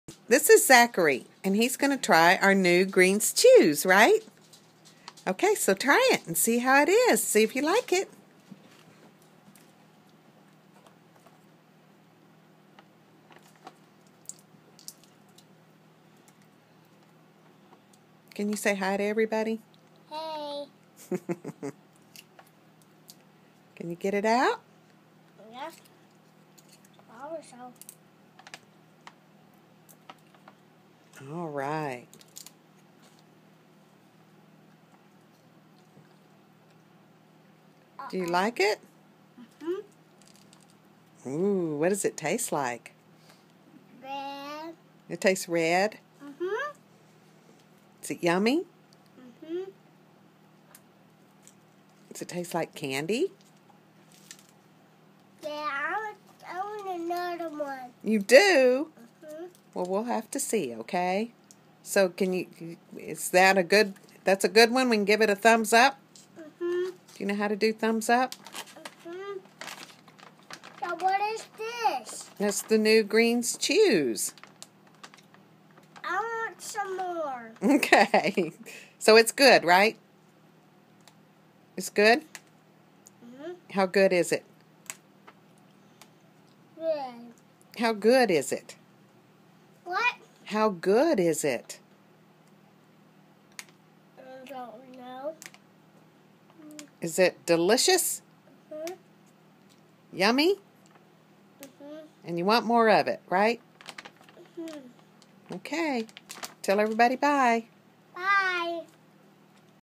Greens Chews taste test
Another satisfied taster of new Greens Chews from It Works Global